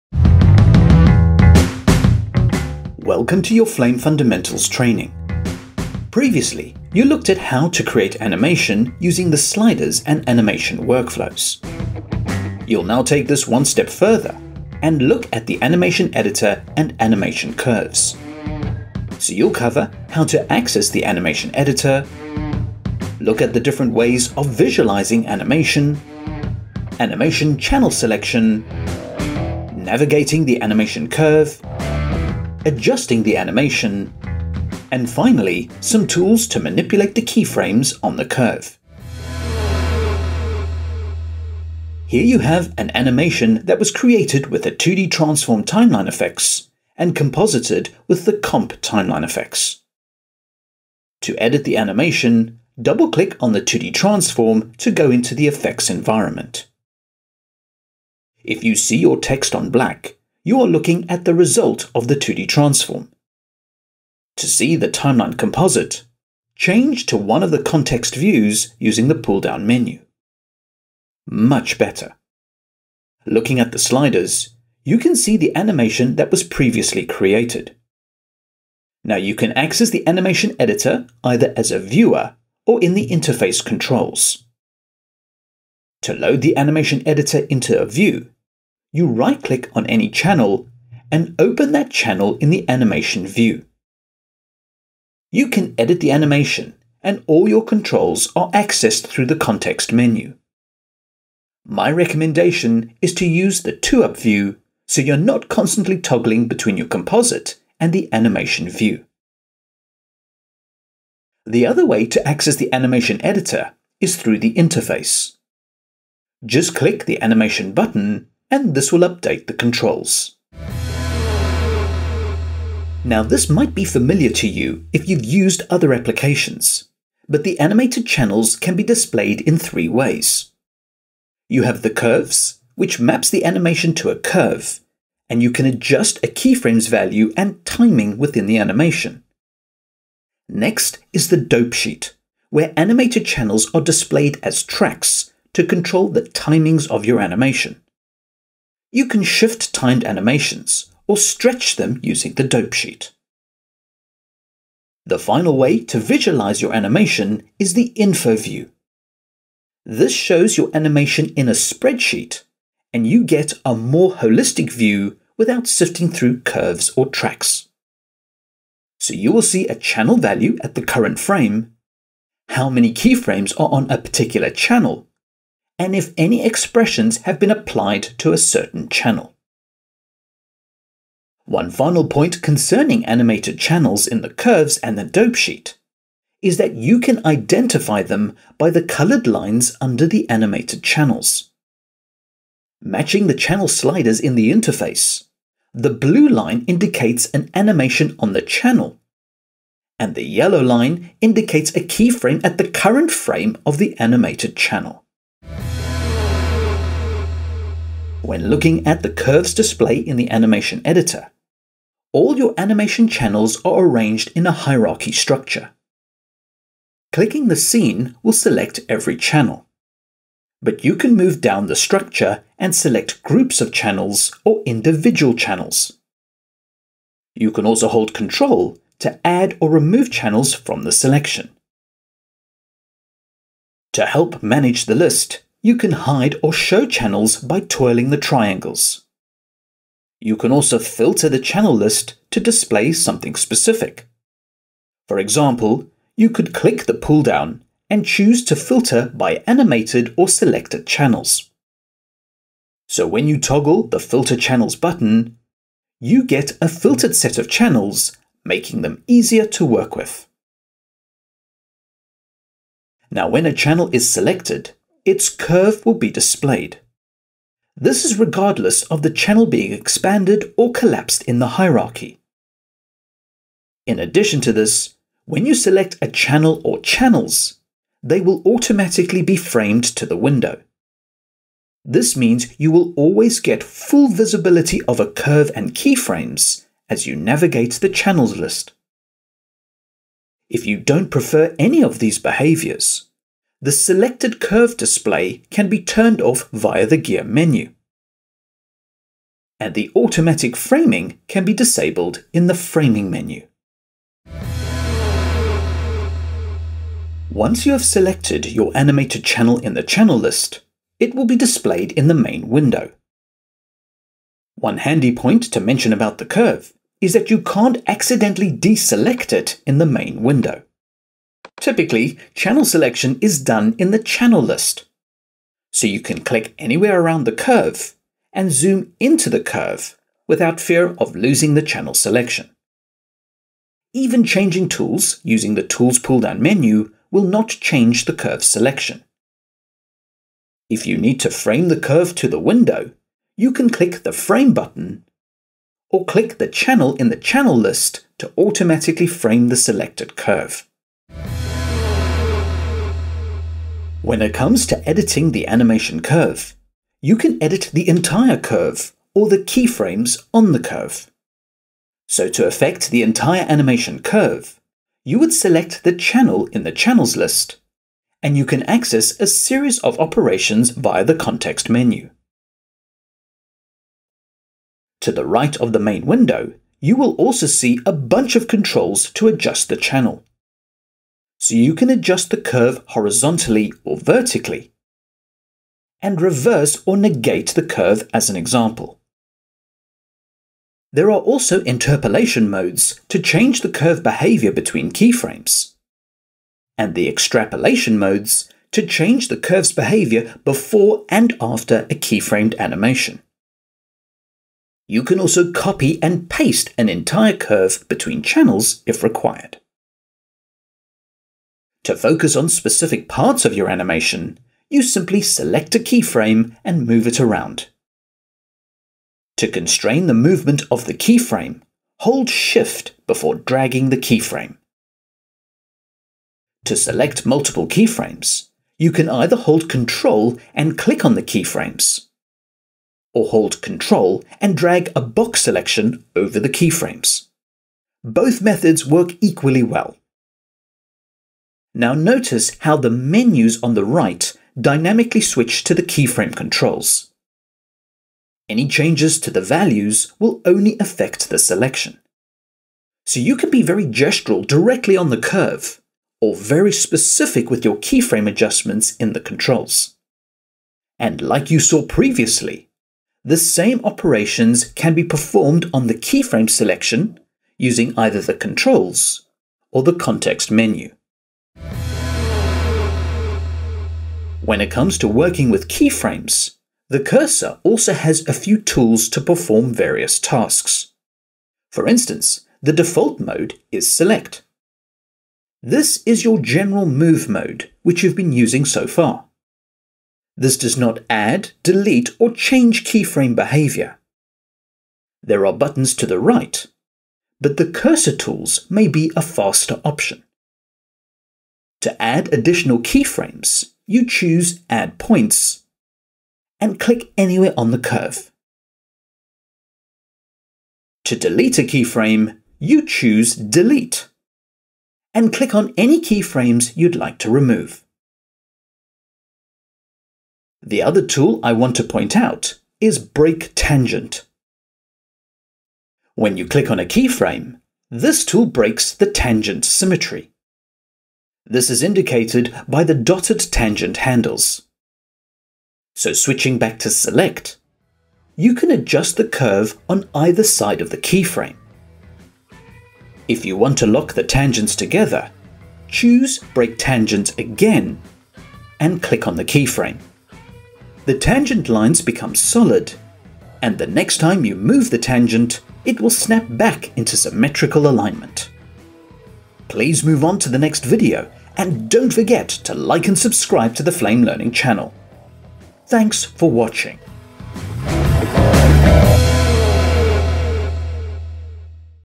Flame Fundamentals - Module 8 - Part 10 - Curves in the Animation Editor
Once you have created any animation in Flame, all the animation is stored in the Animation Editor.  This is displayed as curves on a graph or tracks on a dope sheet.  All the animation can accessed and edited to fine tune all animated channels.

To download the media and follow along, click the link to Module 1 - Part 1 and you will find the download link in the video description

Level: Beginner
Recorded in: Autodesk Flame 2023.1

So in this video, you’ll cover how to access the Animation Editor, look at the different ways of visualising animation, animation channel selection, navigating the animation curve, adjusting the animation and finally some tools to manipulate the keyframes on the curve.

If you wish to follow along, you can download the 30-day Flame trial

or if you are in education, you can sign up for a free license

00:00 - Introduction
00:40 – Accessing the Animation Editor
02:01 – Visualizing Animation
03:26 – Channel Curve Selection
05:05 – Navigating the Curve
05:52 – Editing the Curve
07:52 – Keyframe Tools